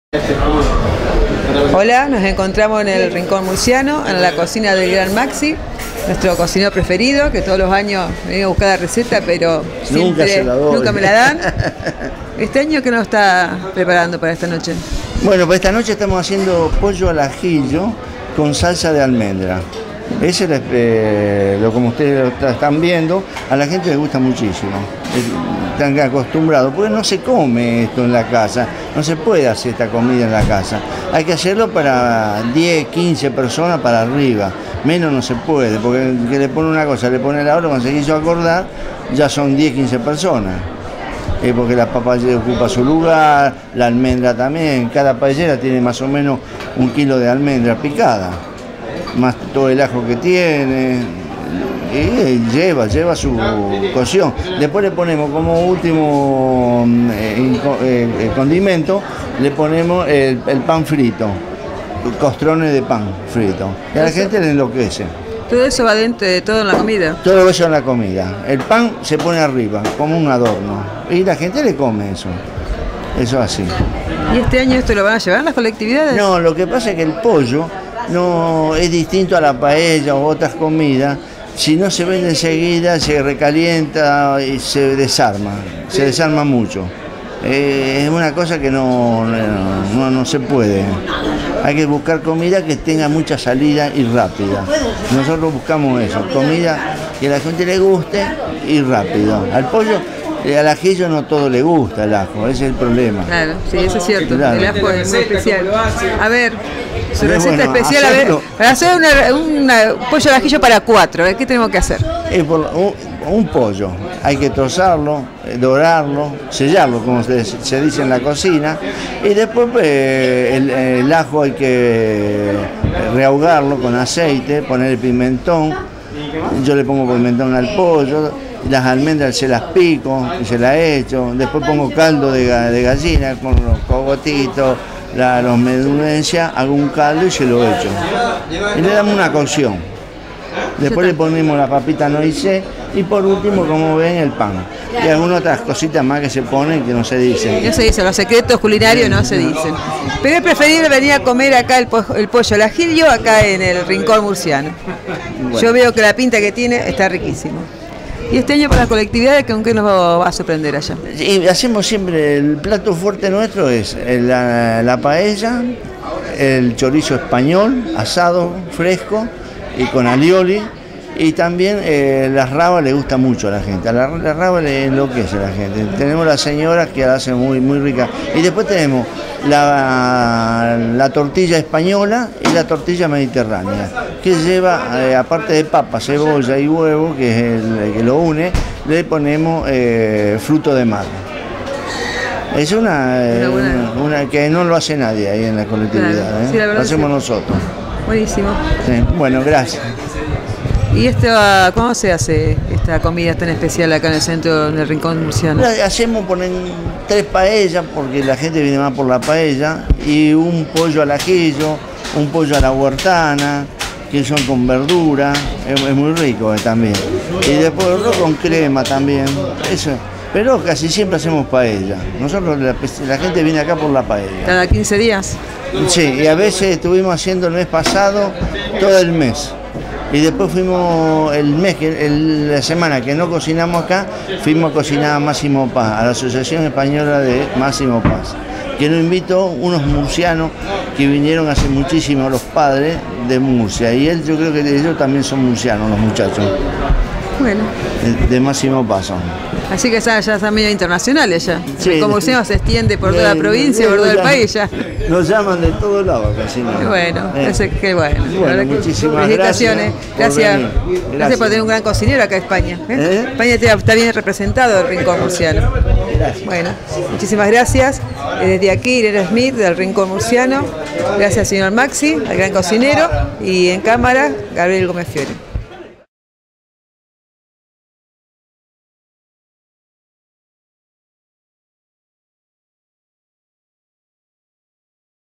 20141018 RINCON MURCIANO Maxi Frugoni Zavala
Entrevistamos a Maxi Frugoni Zavala, Presidente del Rincón Murciano de Rosario, una de las asociaciones de colectividades españolas que se destacan cada año en el Encuentro de Colectividades. Hacen cenas quincenales en su sede de calle Rodríguez al 900 casi San Luis de Rosario.
Mayoritariamente las famosas paellas de Maxi, pero también Pollo al Ajillo, a la crema y otras delicias de la mano de este gran cocinero querido y repetado en la región.
Nota: Irene Schmidt
Cámara: Gabriel Gomez Fiori
Edición: NUEVAREGION.COM